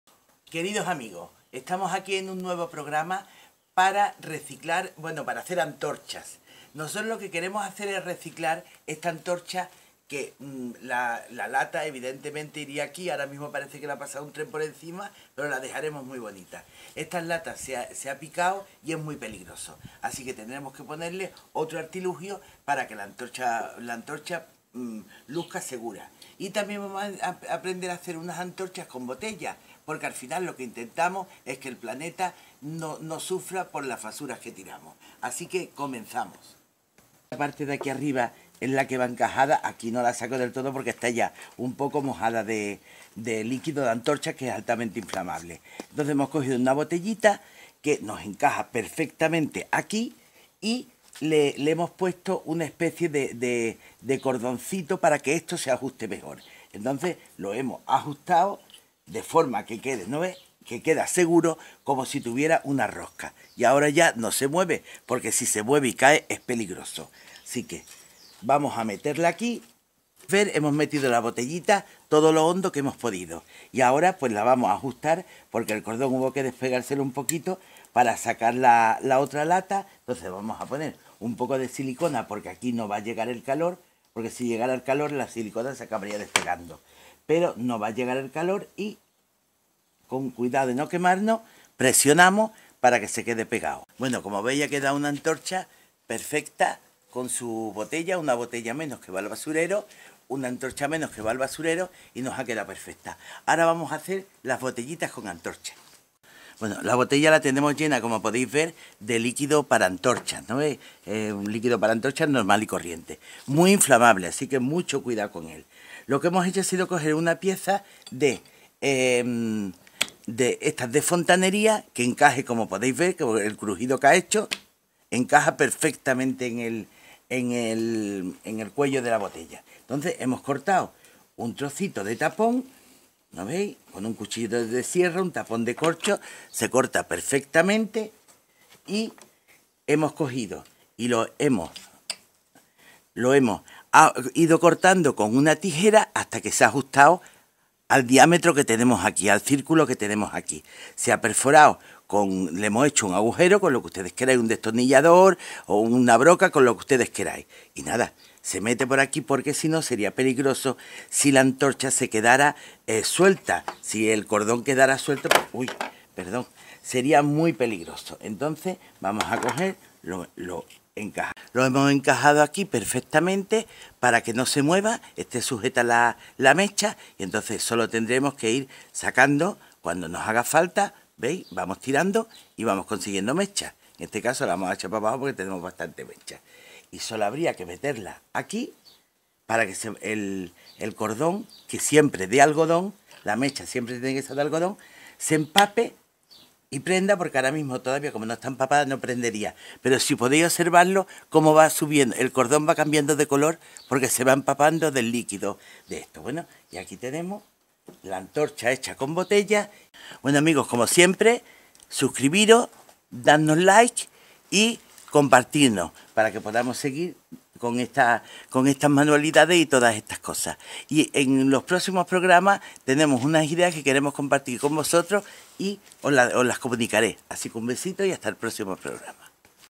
Ideas para hacer antorchas caseras para jardines, balcones y patios
Hoy os damos una idea para reciclar antorchas de jardín deterioradas y otra para crear antorchas caseras, si no tenemos, sencillo, ecológico y económico.